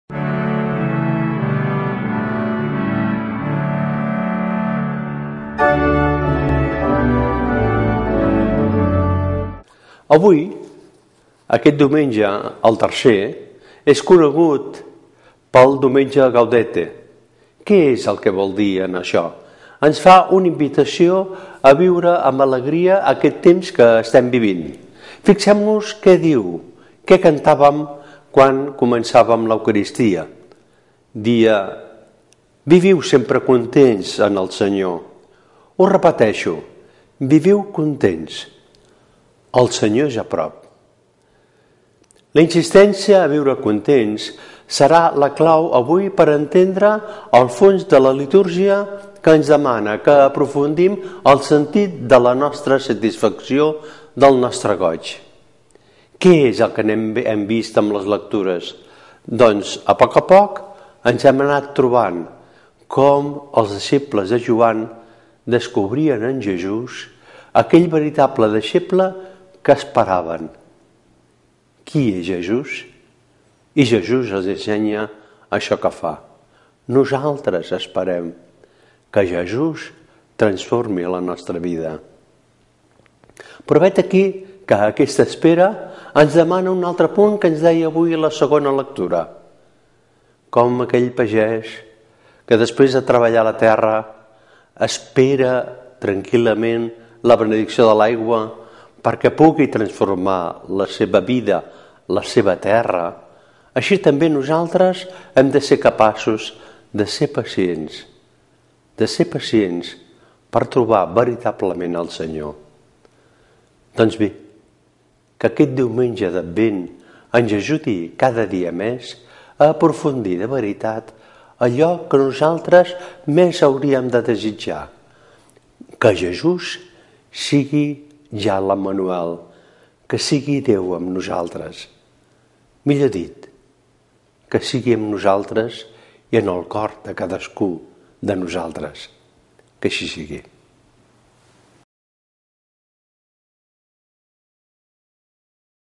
Anem a l'encontre del Senyor: Diumenge III d'Advent III, cicle A
Comentari als textos litúrgics corresponents al Diumenge III d'Advent III, cicle A. pel P. Lluís Planas, monjo de l'Abadia de Montserrat (desembre de 2013)